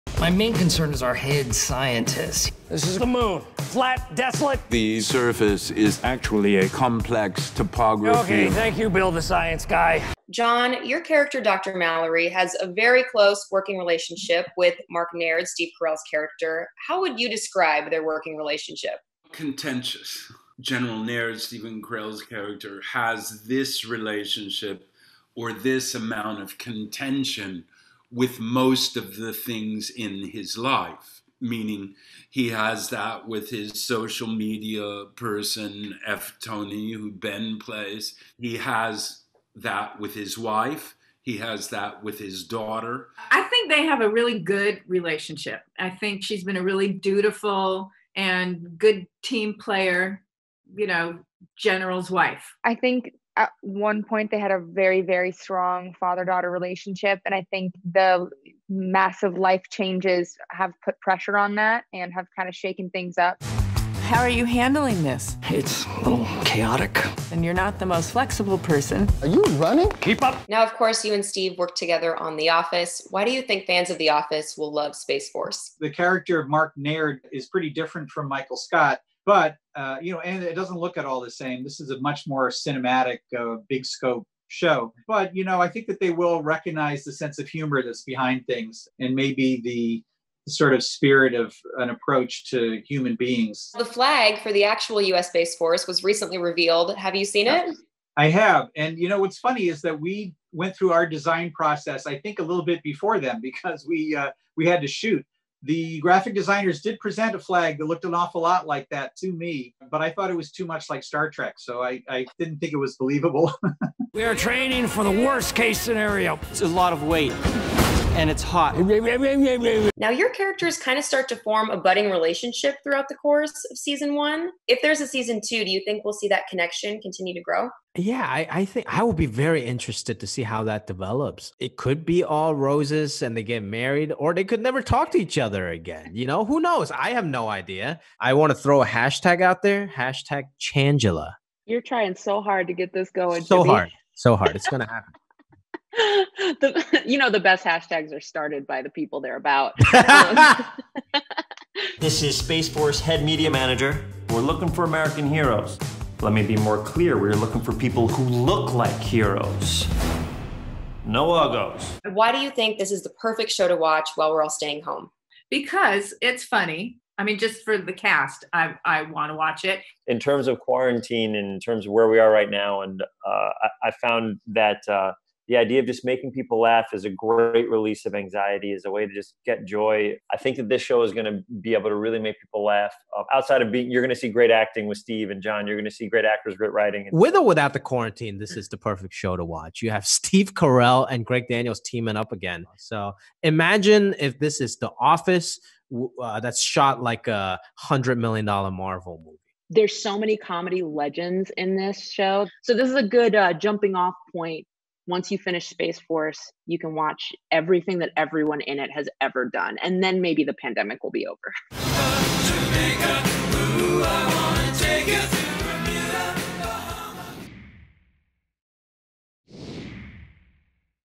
'Space Force' Cast on Why 'Office' Fans Will Love Their Show | THR Interview
'Space Force' stars John Malkovich, Ben Schwartz, Lisa Kudrow, Diana Silvers, Tawny Newsome, Jimmy O. Yang, and co-creator Greg Daniels join The Hollywood Reporter to talk about the highly anticipated Netflix series.